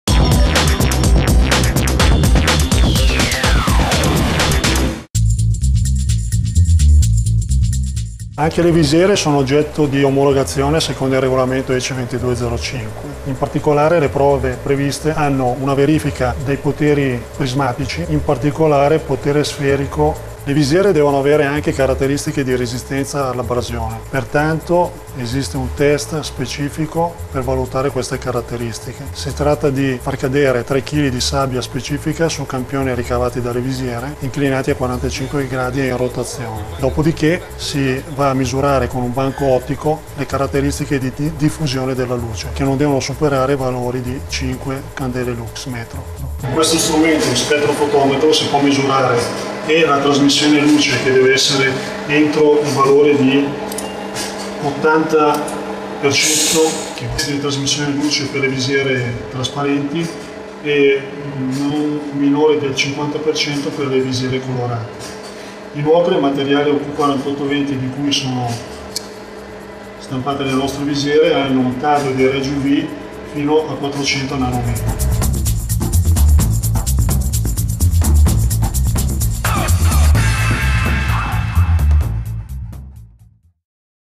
015 SAFETY AND QUALITY I Test Ottici
Giorgio Casali, Nolan Group Homologation and Quality Manager ci spiega come avvengono e a cosa servono i test ottici.